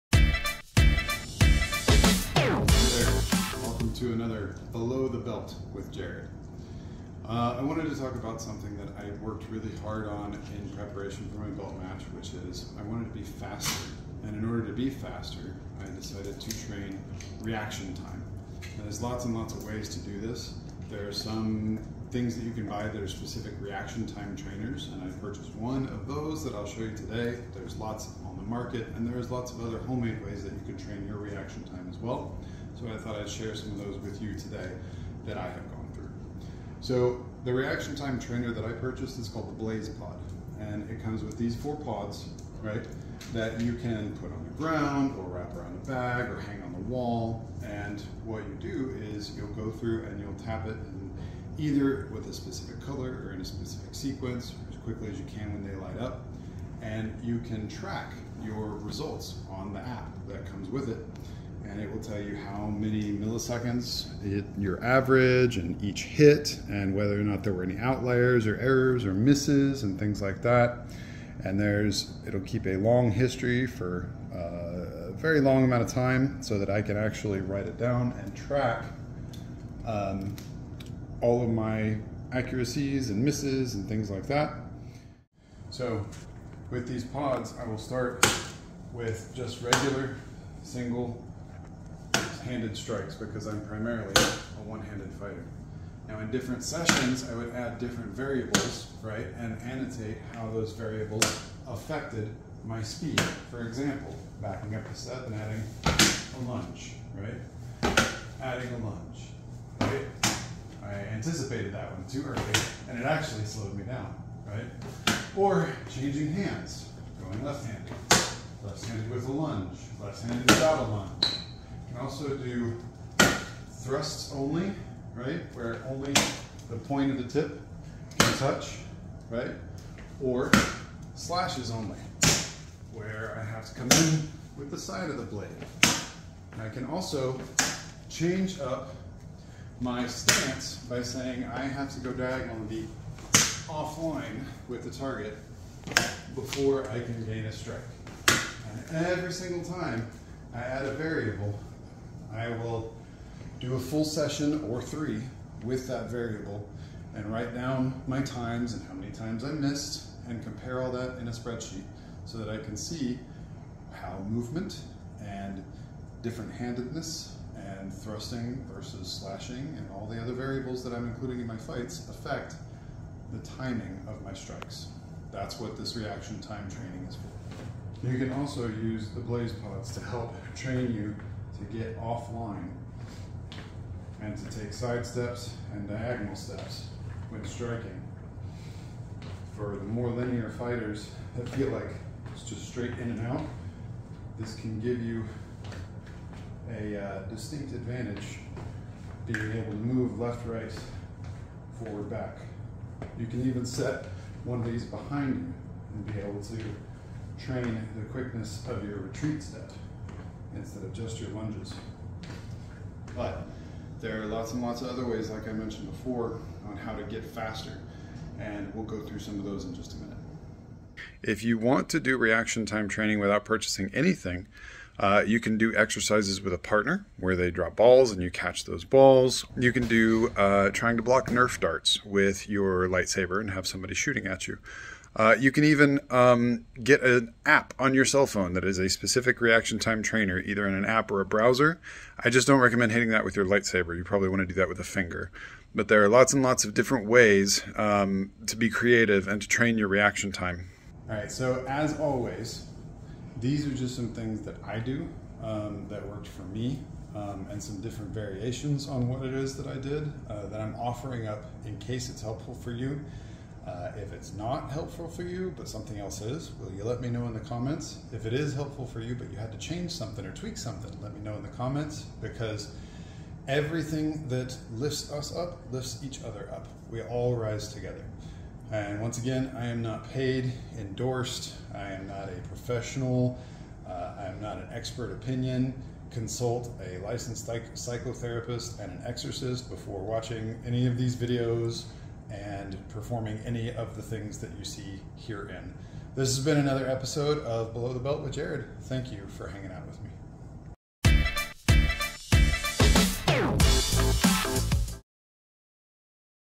Below the Belt with Maximum Effort! Lightsaber Reaction Time Training Challenge.
In this intense lightsaber reaction time training session, join us as we push our Saber skills to the limit! Watch closely as we put our reflexes to the test alongside none other than the reigning World Champion Belt Holder, Maximum Effort. Can we keep up with him? Tune in to find out and learn some tips and tricks along the way to sharpen your own saber skills! Don't miss this epic showdown of speed and precision in the galaxy far, far away! 🚀🔥